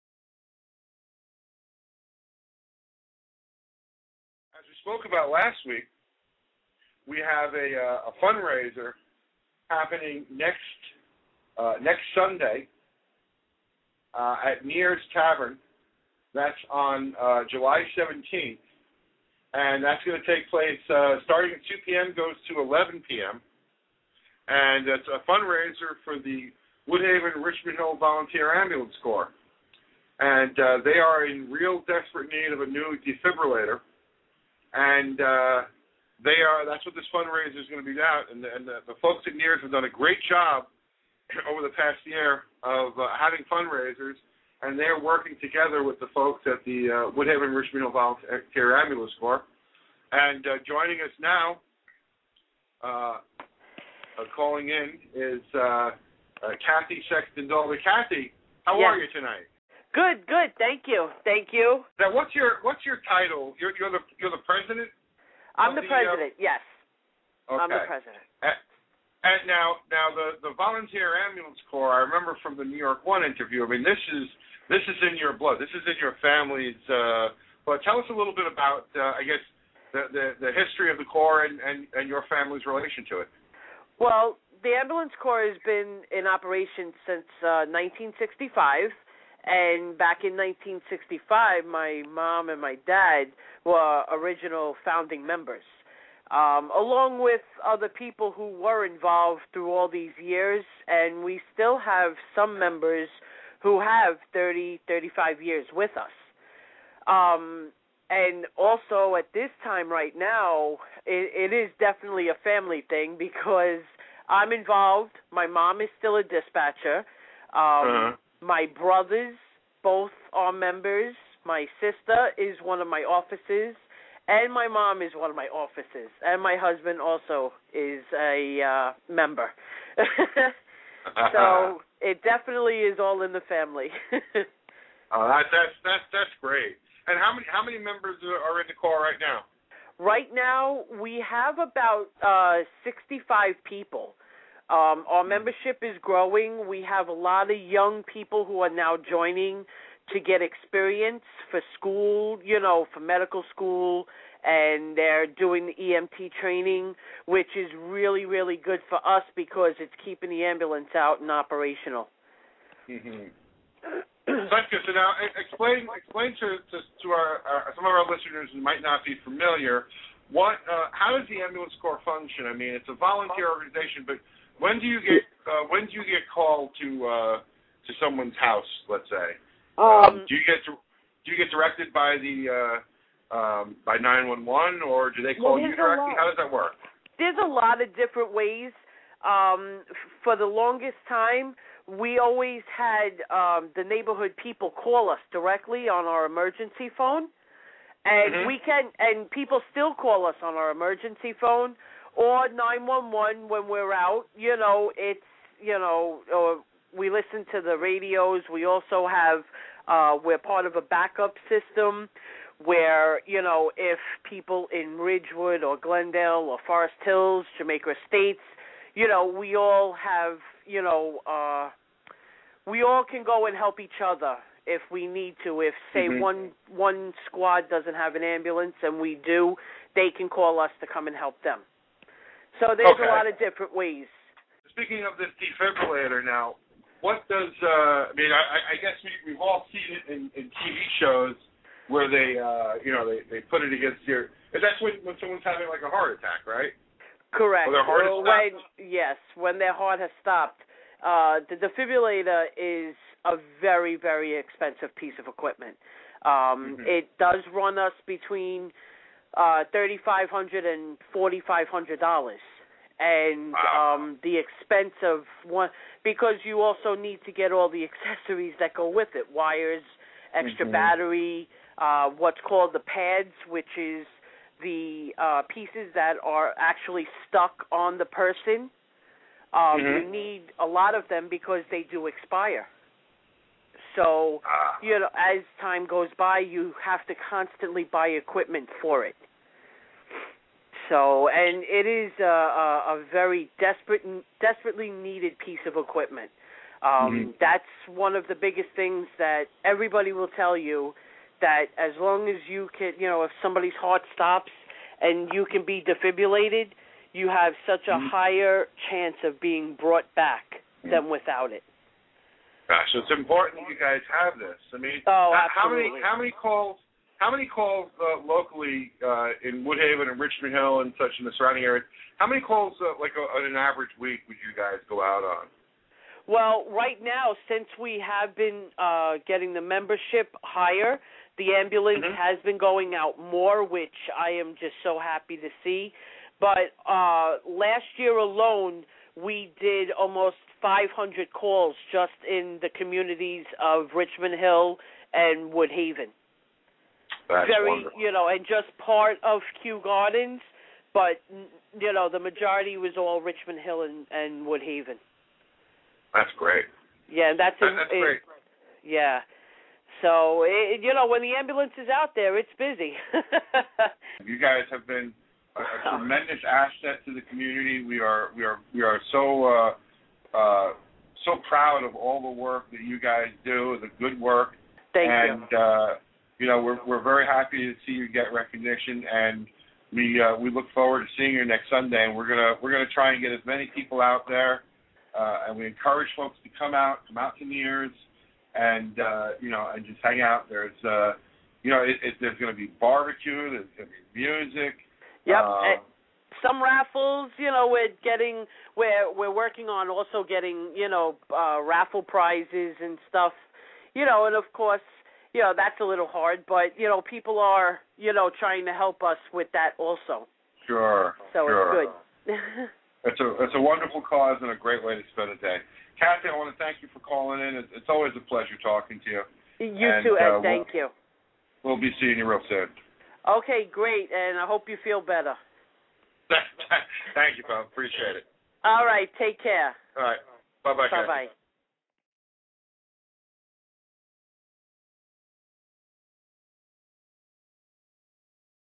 Woodhaven Richmond Hill Volunteer Ambulance Corps Fundraiser July 17
The Woodhaven Richmond Hill Volunteer Ambulance Corps will be holding a fundraiser at Neirs Tavern located at 87-48 78 st Woodhaven Queens on July 17th between 2 and 11 PM.  The Ambulance Corps is in desperate need of new defibrillator, which can save a patient's life when their heart has stopped.  The WRHVAC has served our communities for over 40 years and they need your help.